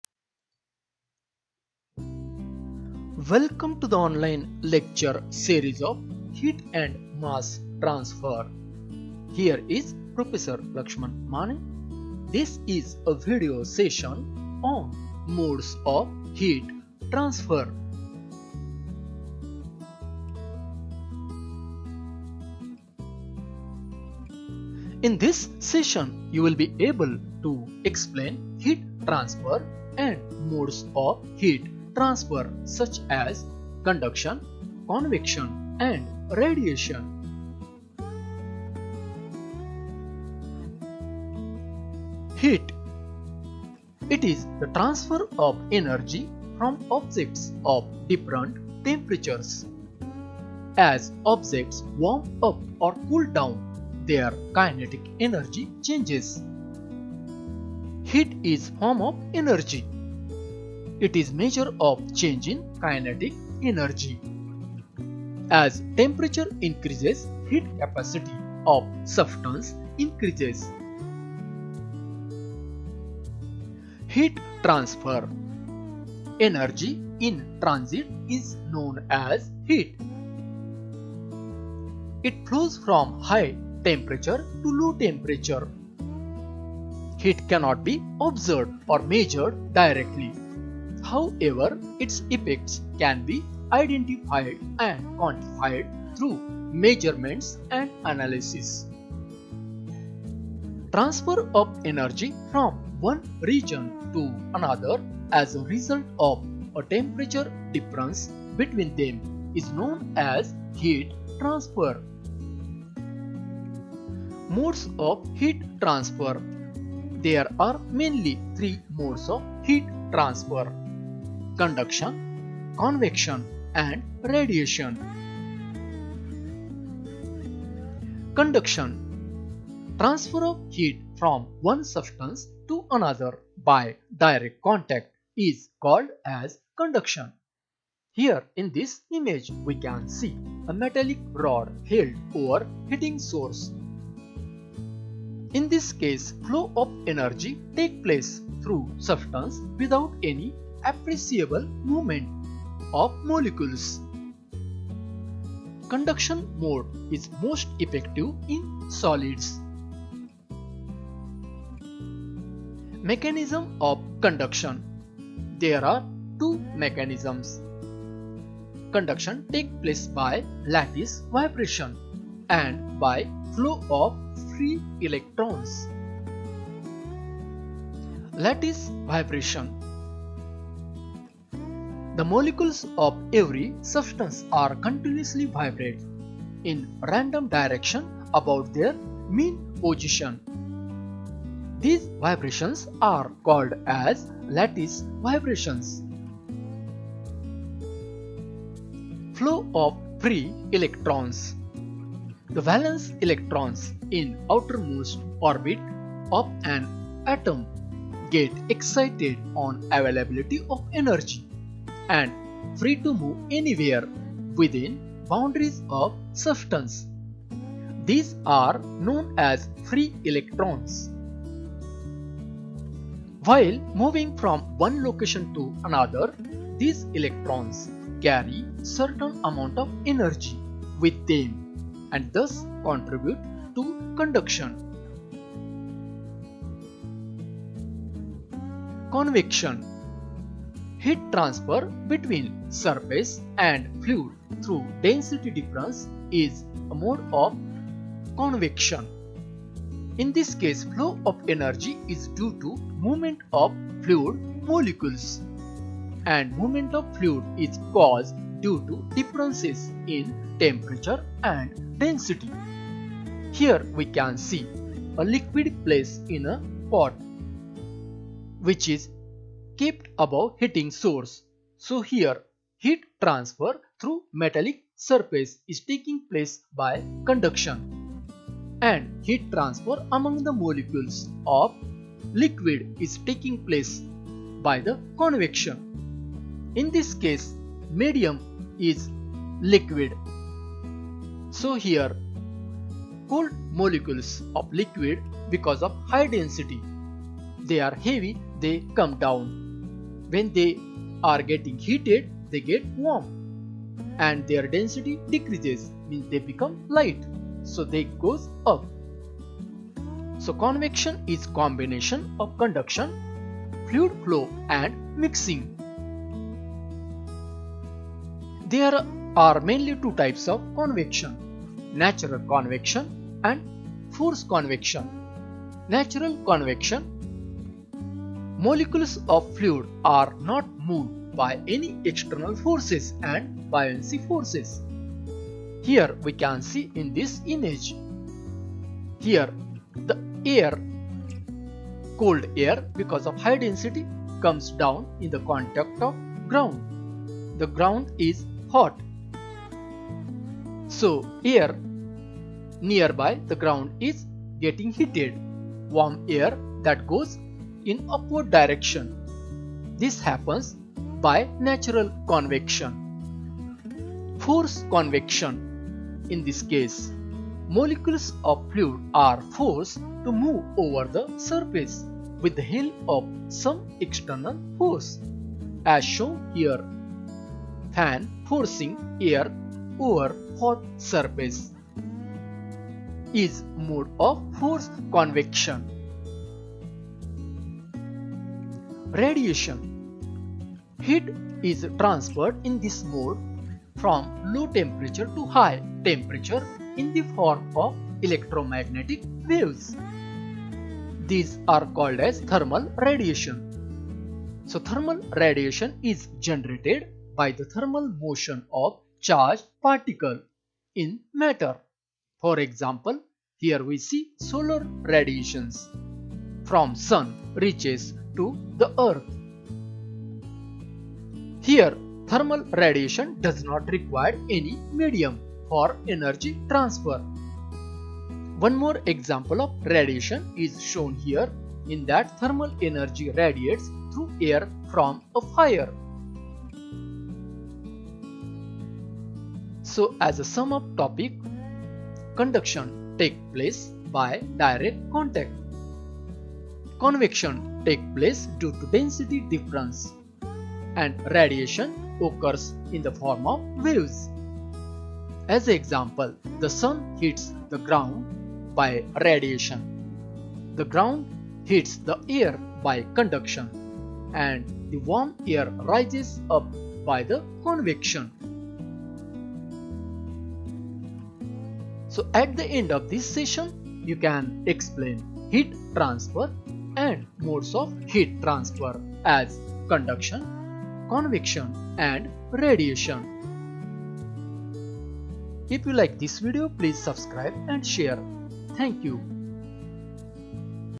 |Modes of Heat Transfer| Conduction, Convection and Radiation|
This video session  is prepared  to make the students conversant with Modes of heat transfer as conduction,   convection and radiation.
[Courtesy: Images]
I acknowledge my sincere thanks to the authors and references, referred here.